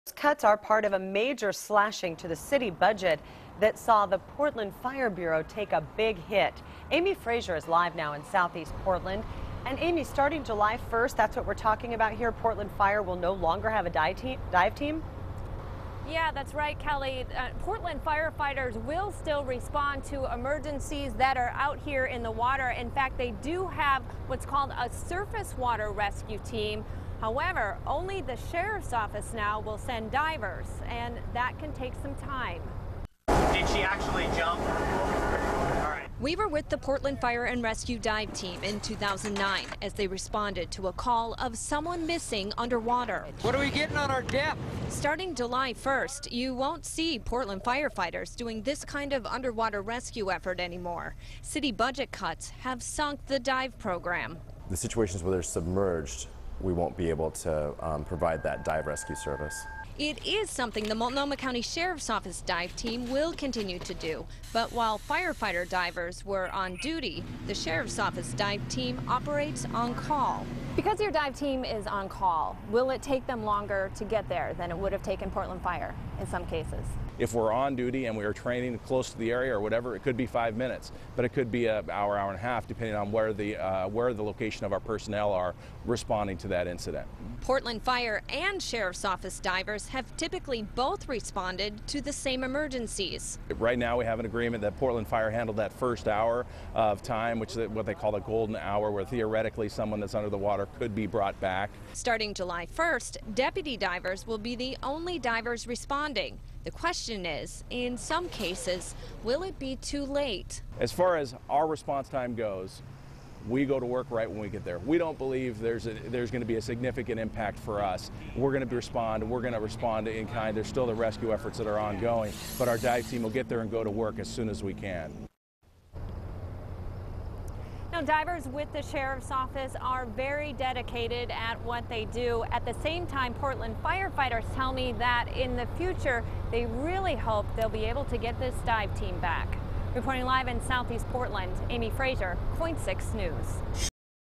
Budget makes PF&R Dive Team pack up gear
Budget cuts mean the PF&R Dive Team will no longer dive, beginning July 1